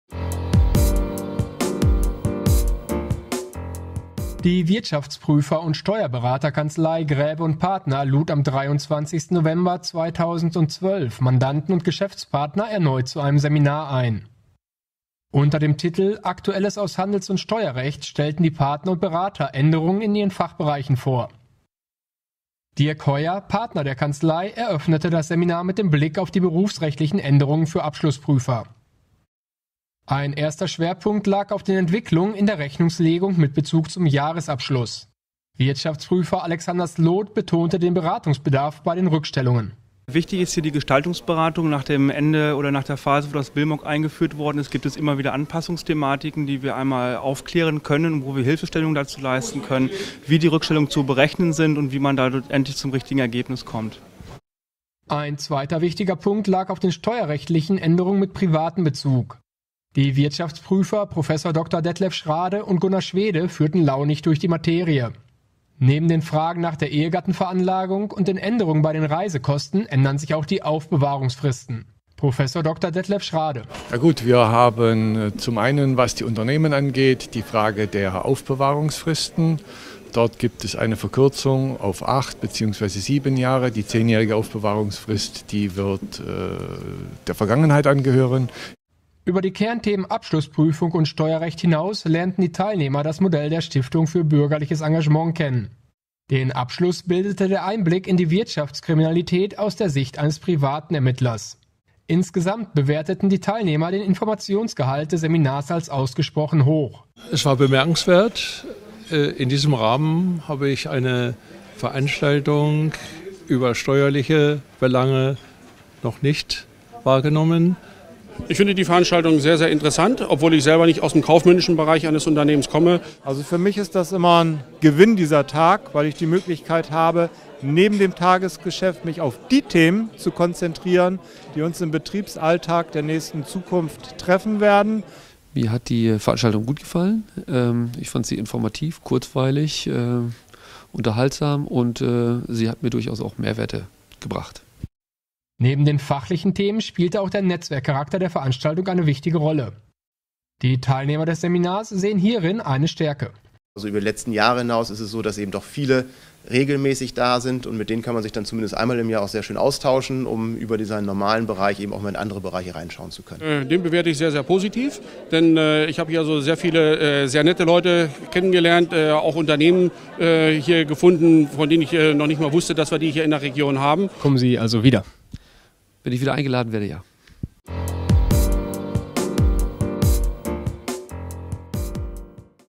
Aktuelles aus dem Steuer- und Handelsrecht, Seminar in Bremen
Gräwe & Partner, Steuerberater und Wirtschaftsprüfer, informierten am 23. November 2012 Mandanten und Geschäftspartner über jüngste Entwicklungen mit Bezug zum Jahresabschluss